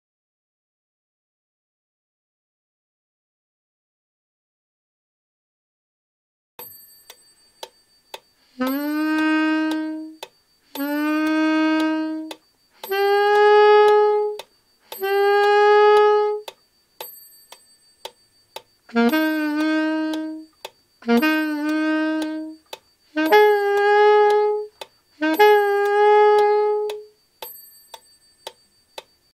43 ベンドの練習
教則本『DVDで今日から吹ける！ かんたんアルト・サックス New Edition』および『DVD＆CDでよくわかる！ はじめてのアルト・サックス New Edition』の対応動画です。

『DVDで今日から吹ける！ かんたんアルト・サックス New Edition』

『DVD＆CDでよくわかる！ はじめてのアルト・サックス New Edition』